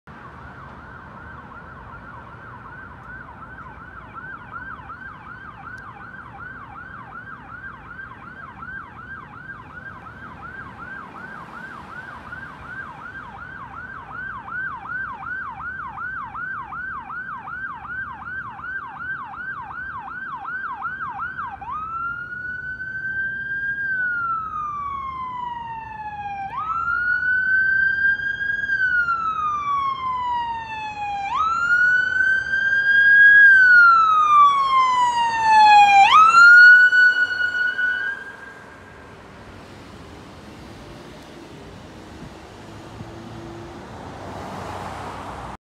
Medic 1222 Transporting
Benton County Fire Protection District 2 Medic 1222 (with a peace sign) Transporting to Kadlec Regional Medical Center, Code 3 from a Med 2 at Hillview Mobile Home Park on 14th St, Station 210s area, Benton City. Medic 1222 requested a medic out of Richland to do a rendezvous with them near the Milepost 1 or Milepost 2 of I-82 near the I-182 interchange. Medic 1724 was dispatched and was advised by Medic 1222 that they had a 4 year old male with a decreased level of consciousness. Medic 1724 and Medic 1222 did a rendezvous and Medic 1222 continued to Kadlec.

This response was taken on August 31, 2022.